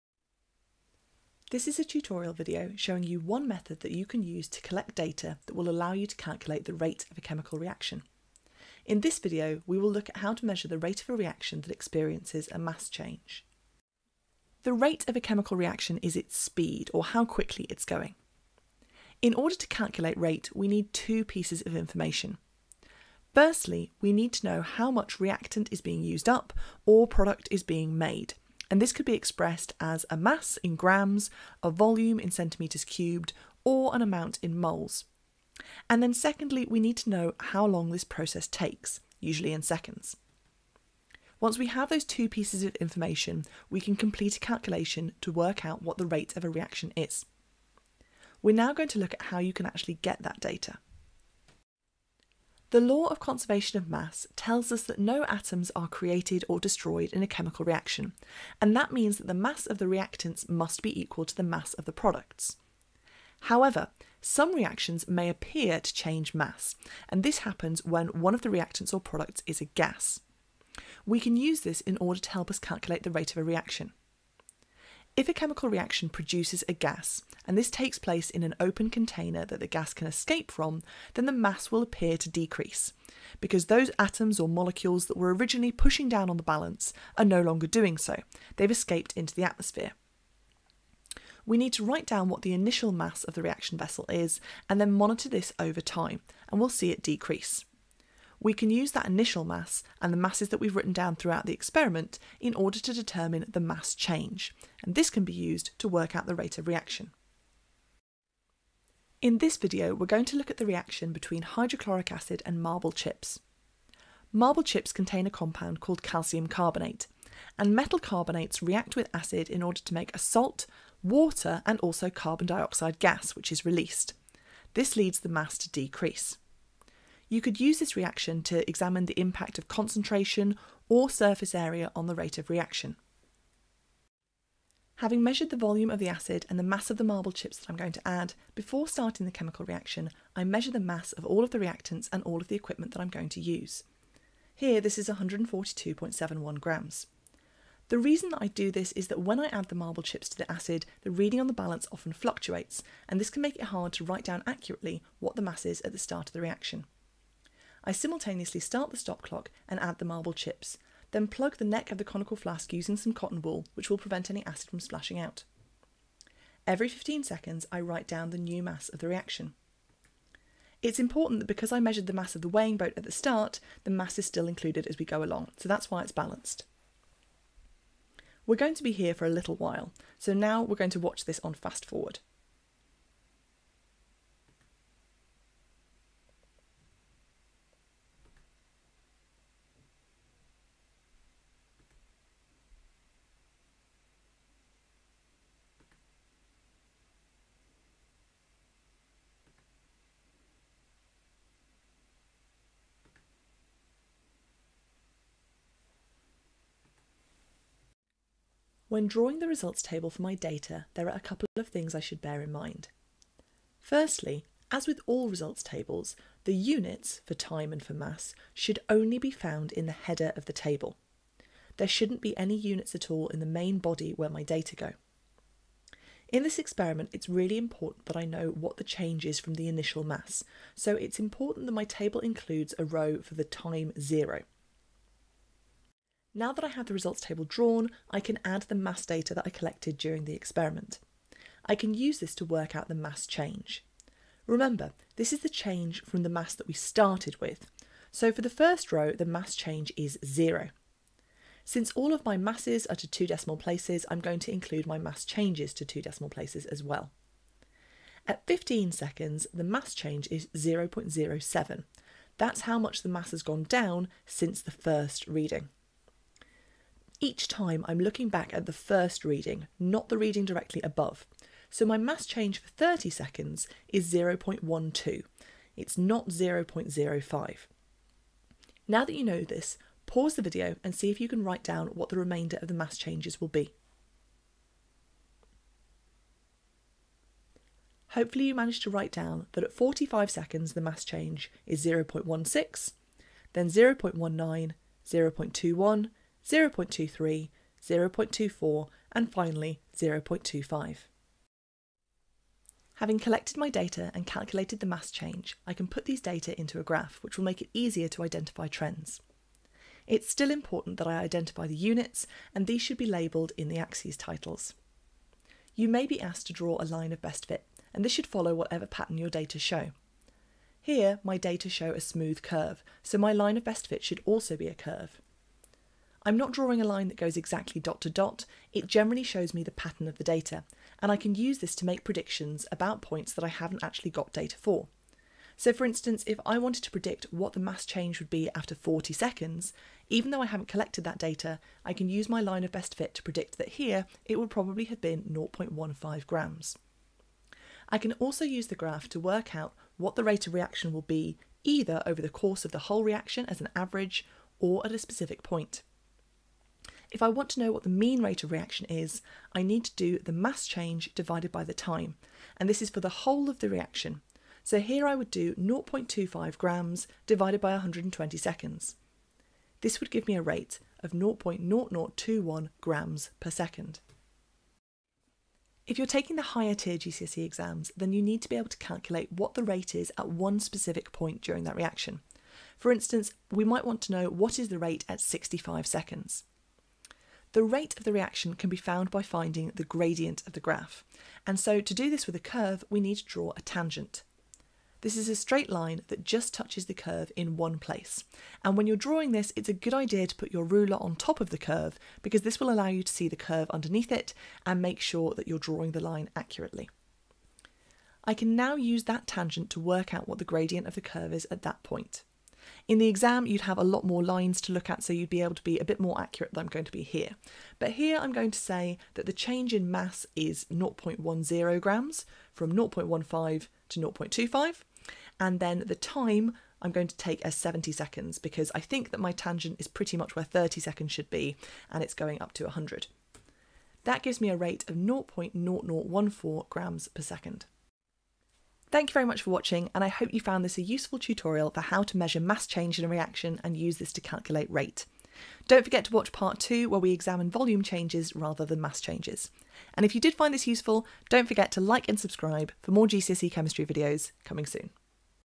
Rates of reaction using mass change
This is a practical tutorial video, showing you how to collect the data to calculate rate of reaction, from a reaction where the mass changes.

This is covered in section 6.1.1 Calculating rates of reactions of the AQA GCSE Chemistry (8462) and Combined Science (8464) specifications